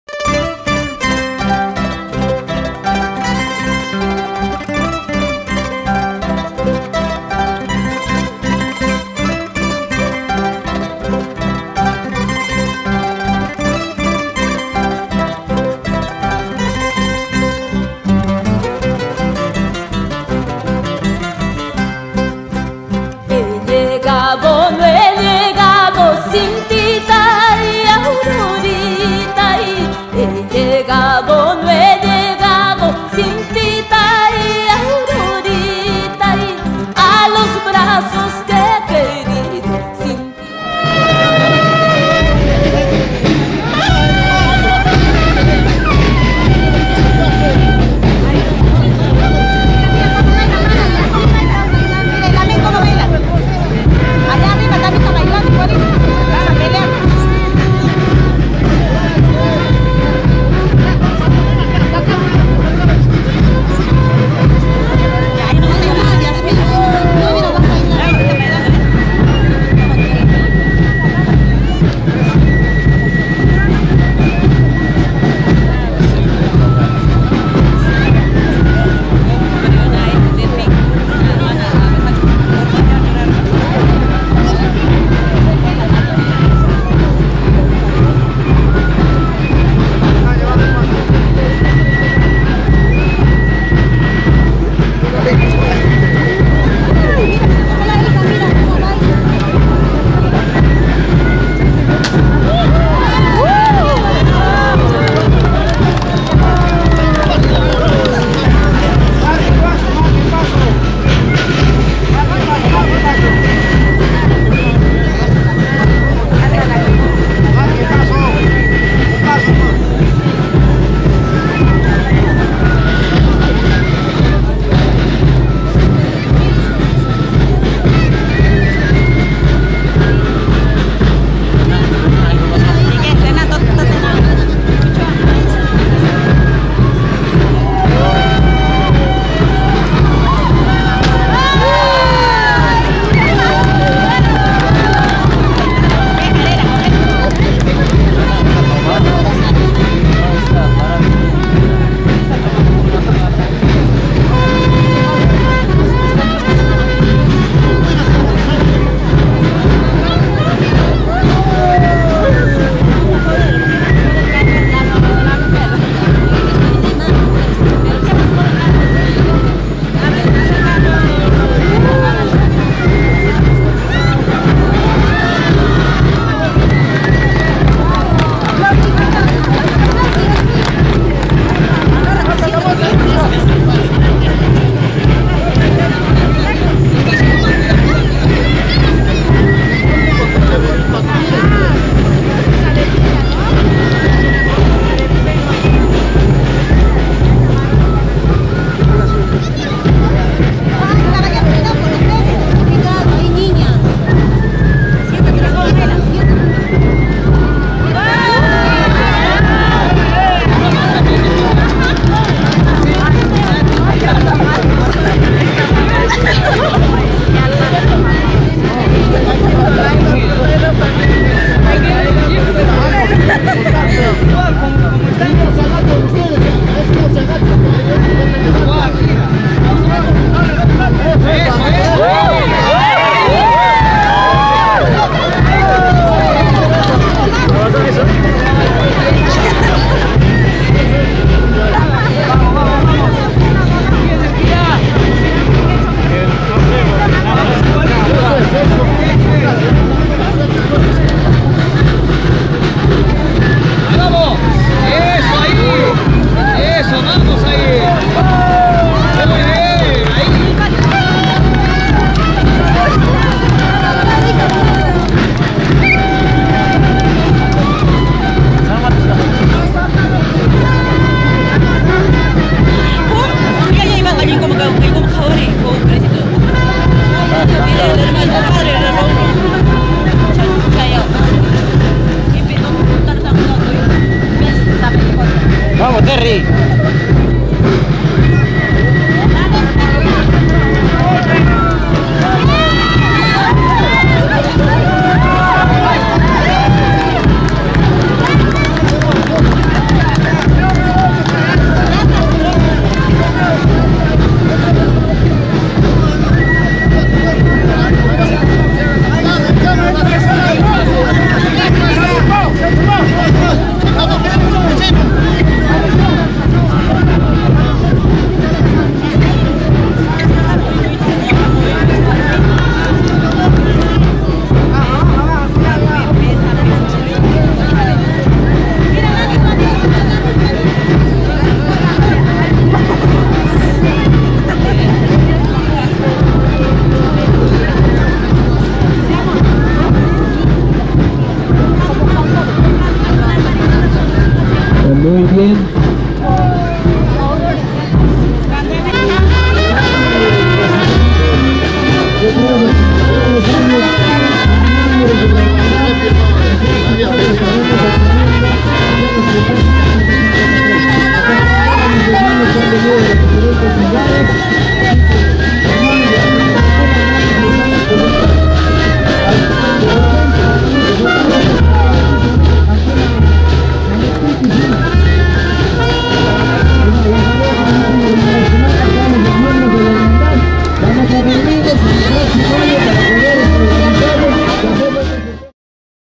PELEA DE SANTOS - SAN FRANCISCO DE ASIS VS SAN MARCOS
LOS SANTOS, SAN FRANCISCO DE ASIS DE OROPESA VS  SAN MARCOS DE TOTORA- EN UN ENCUENTRO  DE DEVOCIÓN Y FE, PROVINCIA DE ANTABAMBA, REGIÓN APURIMAC